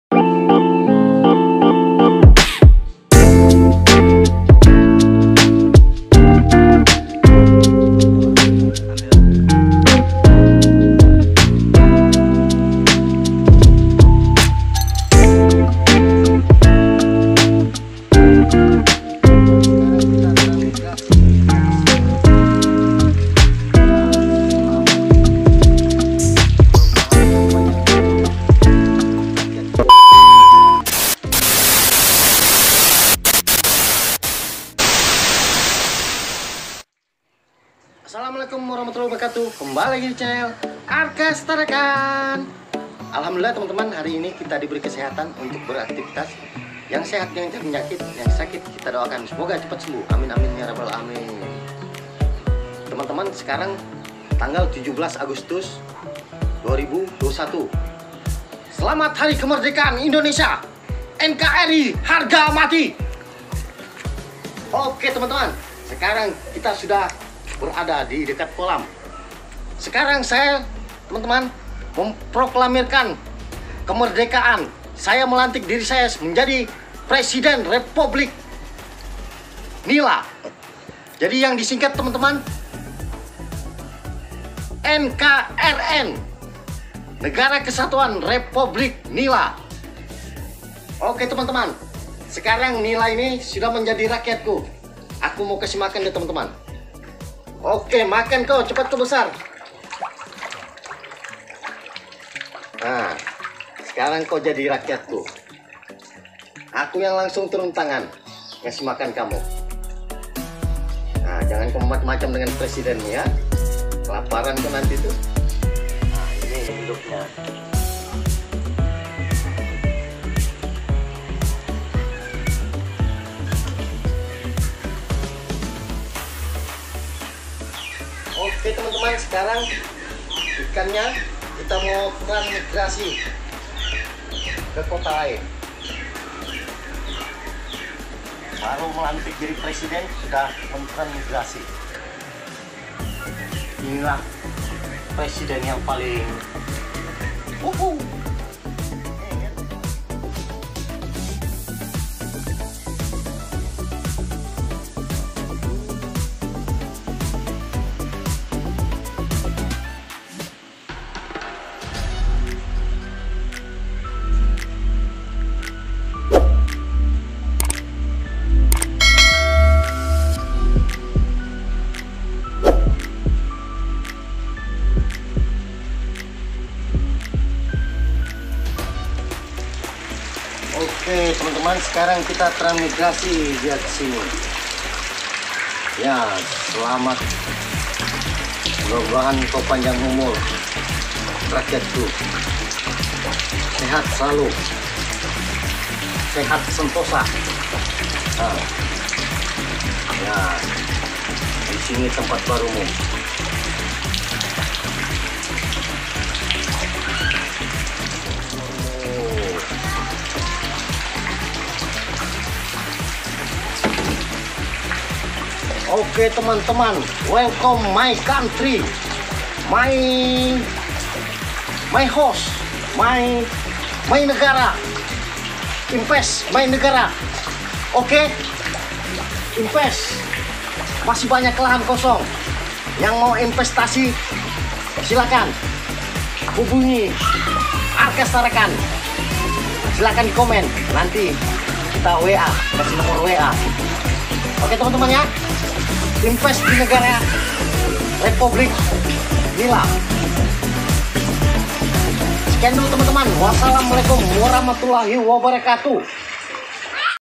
budidaya ikan nila kolam beton/kota Tarakan.
lokasi alamat budidaya ikan nila jl sebengkok AL.Rt 29
jual bibit ikan nila.

ARKAS TARAKAN,🙏🙏🙏